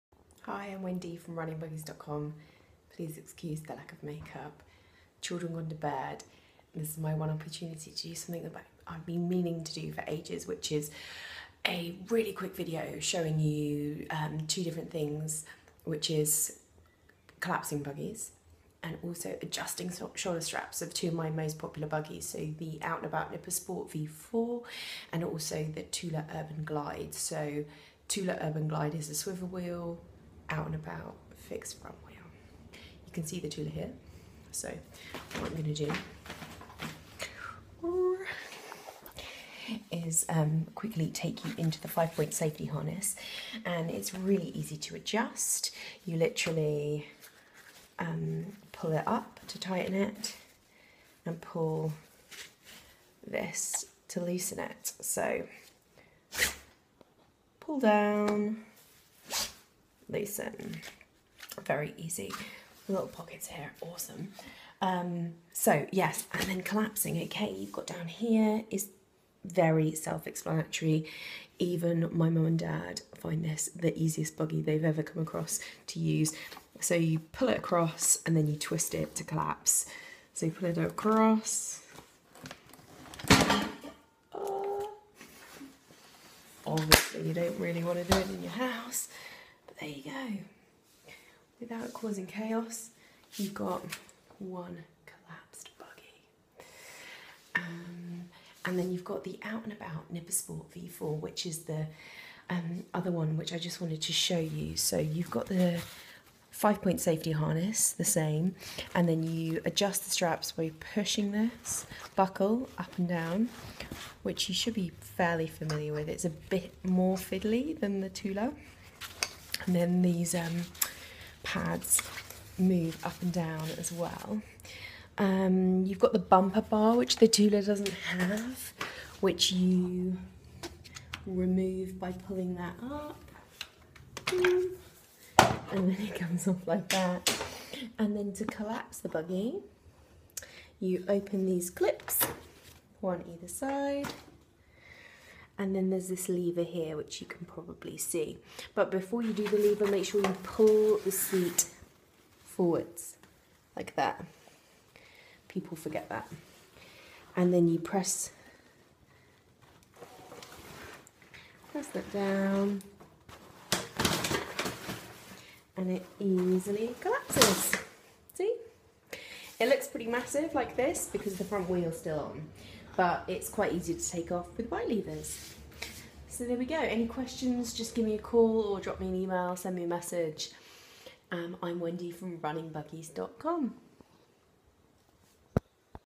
Running buggies Thule & Out n' About straps & collapsing
Comparison of the Out n' About Nipper Sport v4 and the Thule Urban Glide shoulder strap adjustments and collapsing for storage